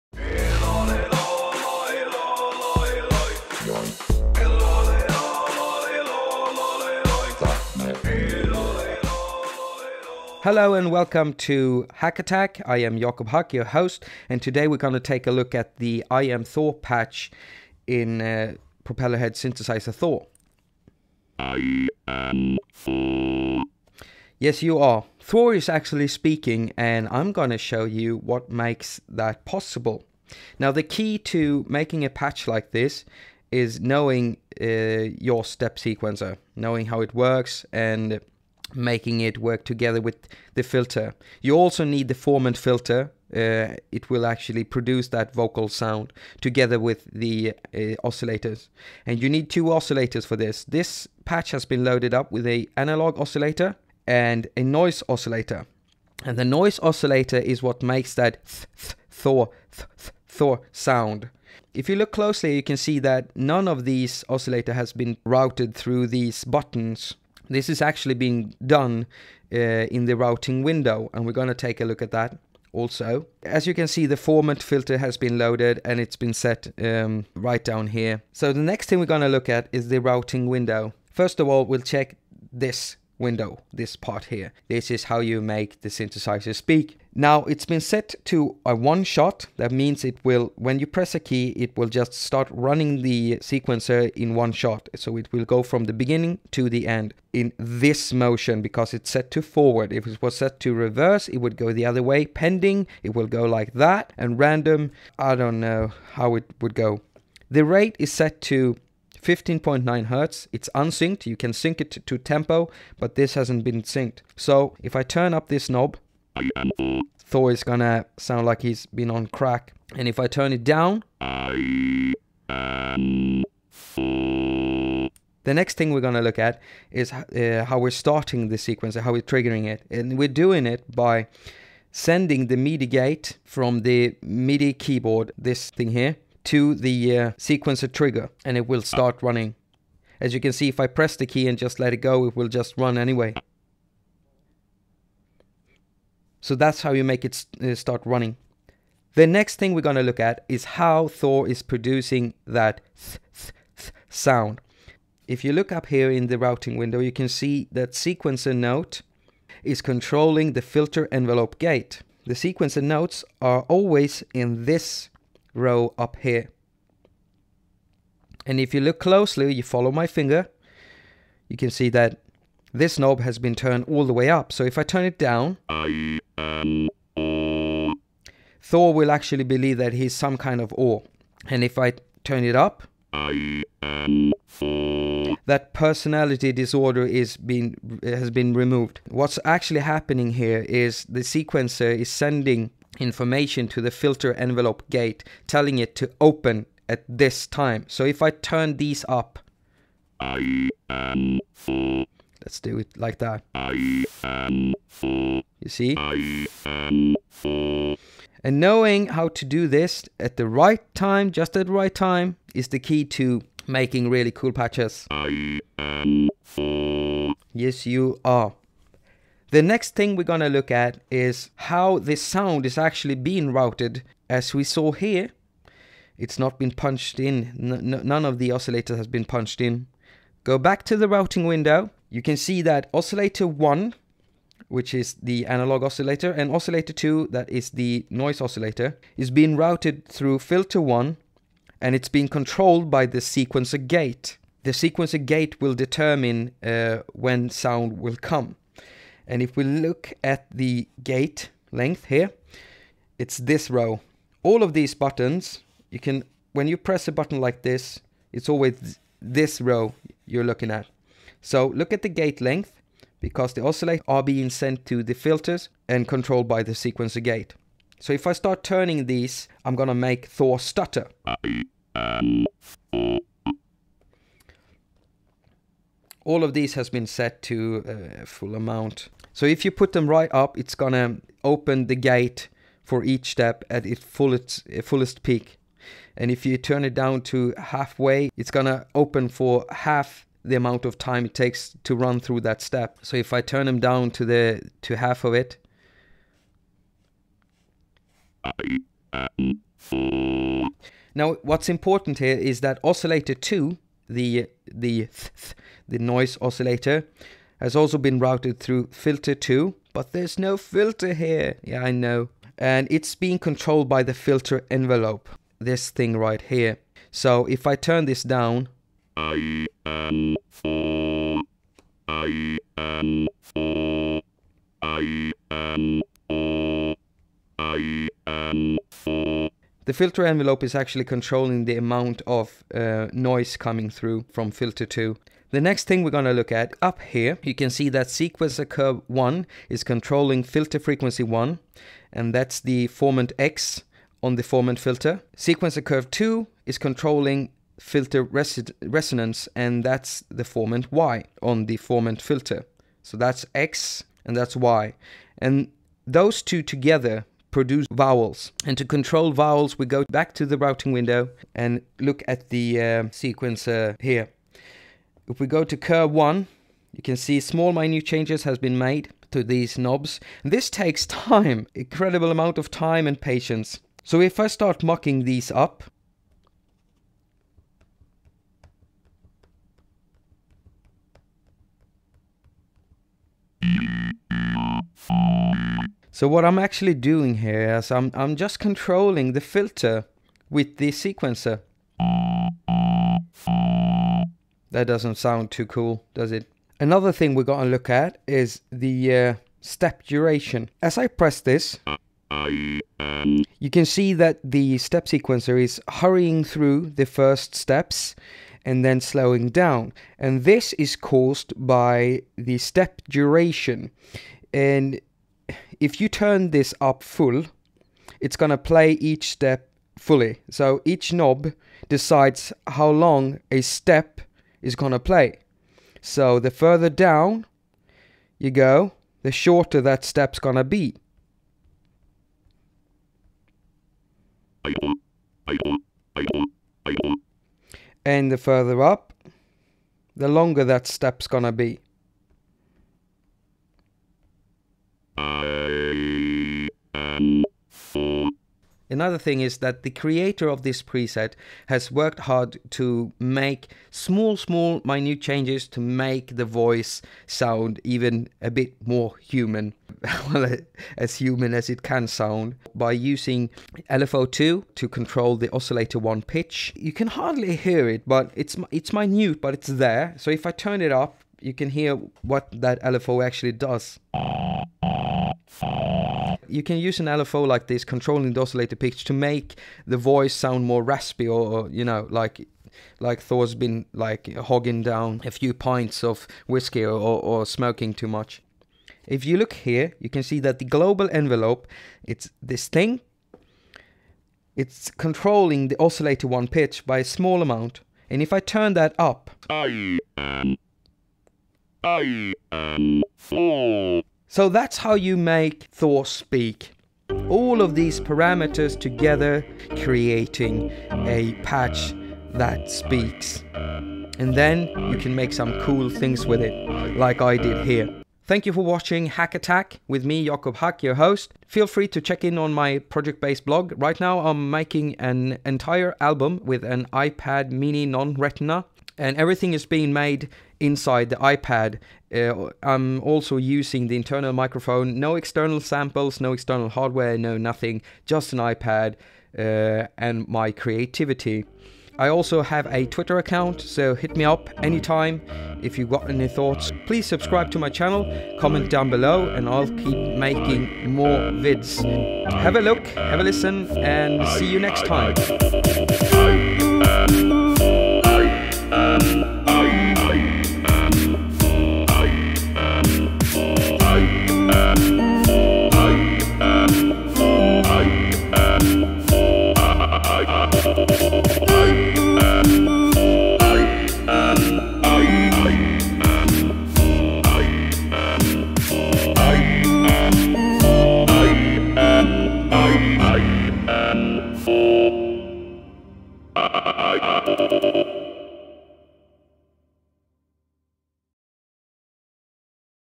I am Thor preset Explained │ Propellerhead Thor for iPad │ Tutorial - haQ attaQ 2
In this video I'm explaining how the "I am Thor" preset in Propellerheads synth "Thor" works.